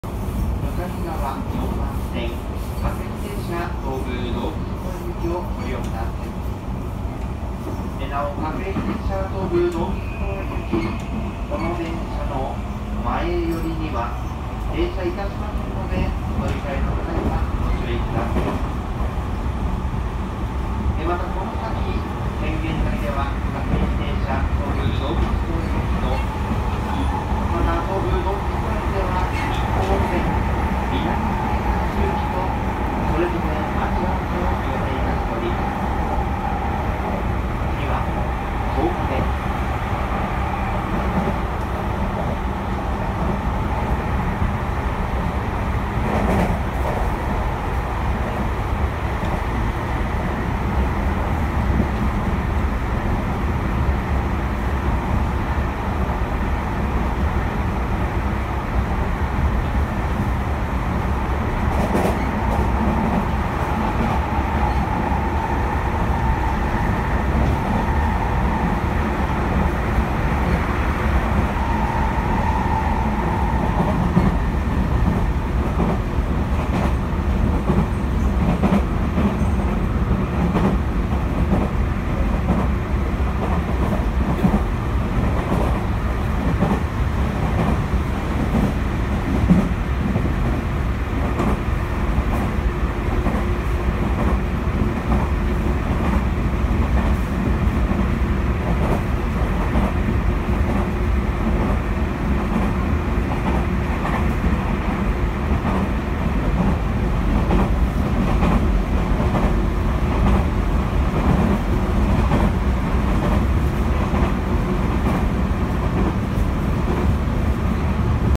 2021-09-19 竹ノ塚駅高架化工事の様子(下り急行線)
下り急行線から撮影。

撮影車両：東急2134F2934